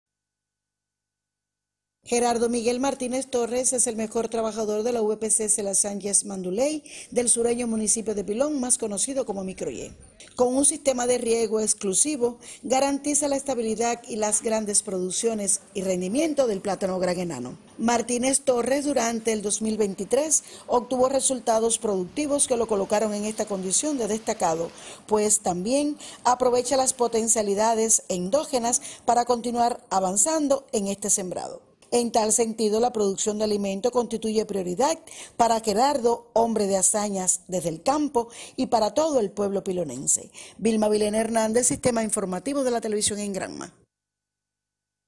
Sostenido y Elevado rendimiento agrícola del plátano fruta de la variedad Gran Enano
El sostenido y elevado rendimiento agrícola del plátano fruta de la variedad Gran Enano, sobresale entre los exponentes del municipio de Pilón, ese es el saldo del esfuerzo de todos los trabajadores, quienes están vinculados a los resultados finales de la producción; son disciplinados en el cumplimiento del programa de atención al cultivo, y conservan el sistema soterrado de riego Microjet,